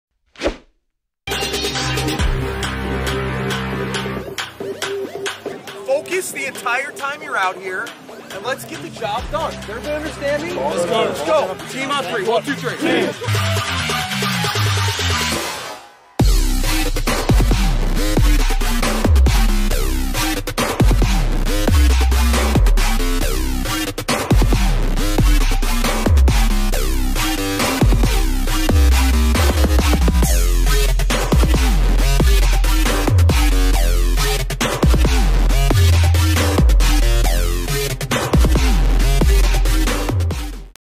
LaxRatz Drone | Agoura/Huntington @ Redondo Union High School
LaxRatz.com drone shots from Redondo Union High School.